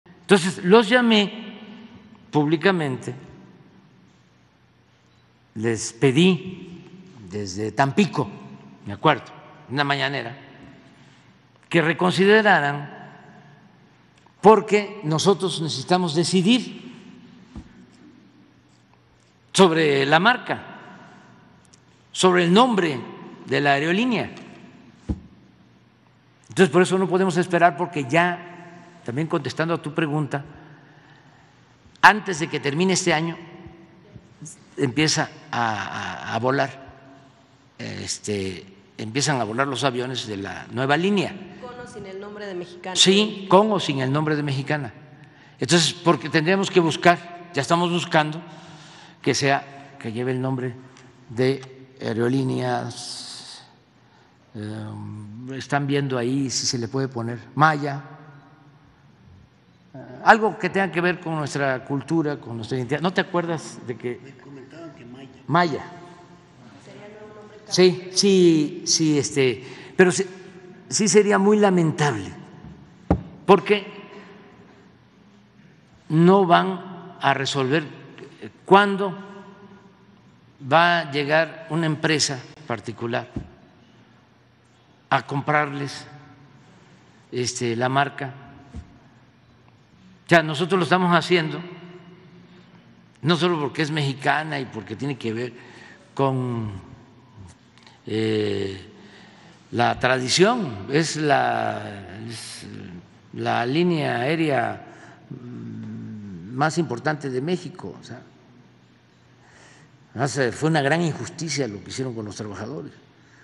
AMLO propone nombrar Maya a aerolínea si no llegan a acuerdo con trabajadores de MexicanaDeAviación
López Obrador propone nombrar Maya a la nueva aerolínea si no llega a un acuerdo con los trabajadores de la desaparecida